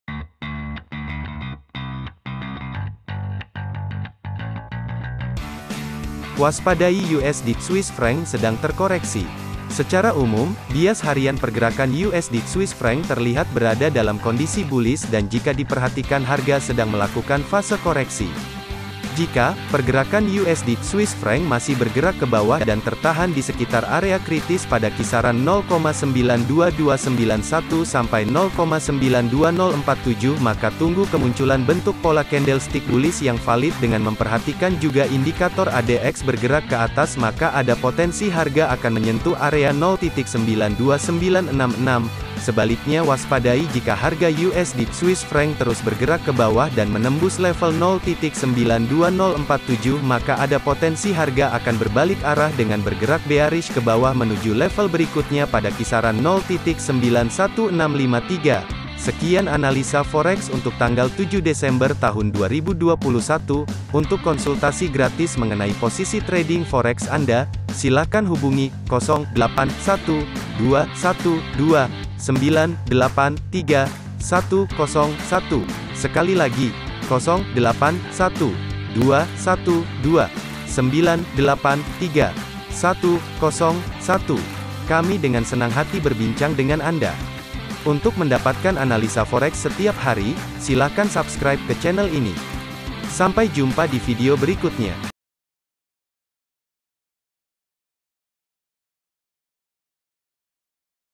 🔴 LIVE: Analisa Forex Hari Ini 7 Desember 2021 Waspadai USD-CHF Sedang Terkoreksi
"Analisa Forex Hari Ini 7 Desember 2021 Waspadai USD-CHF Sedang Terkoreksi